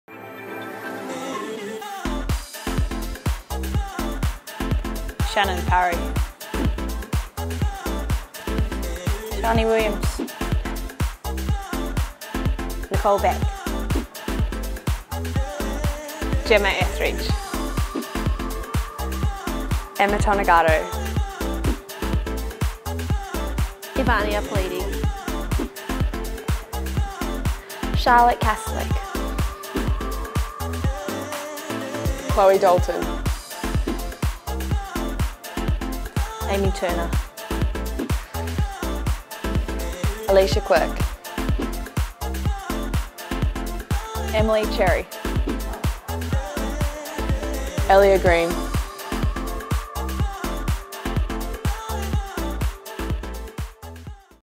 Sevens: Women's squad announcement for Dubai!
Women's Sevens coach Tim Walsh has named his side for Dubai - with a star laden team for the series opener!
1. Shannon Parry. Place of Birth: Brisbane, QLD
2. Sharni Williams. Place of Birth: Batlow, NSW. Hometown: Canberra, ACT
3. Nicole Beck. Place of Birth: Bulli, NSW
4. Gemma Etheridge. Place of Birth: Tamworth, NSW. Hometown: Toowoomba, QLD
5. Emma Tonegato. Place of Birth: Wollongong, NSW
6. Evania Pelite. Place of Birth: Brisbane, QLD
7. Charlotte Caslick. Place of Birth: Brisbane, QLD
8. Chloe Dalton. Place of Birth: Singapore. Hometown: Mona Vale, NSW
9. Amy Turner. Place of Birth: Tokoroa, NZ
10. Alicia Quirk. Place of Birth: Wagga Wagga, NSW
11. Emilee Cherry. Place of Birth: Roma, QLD. Hometown: Toowoomba, QLD
12. Ellia Green. Place of Birth: Suva, Fiji. Hometown: Melbourne, VIC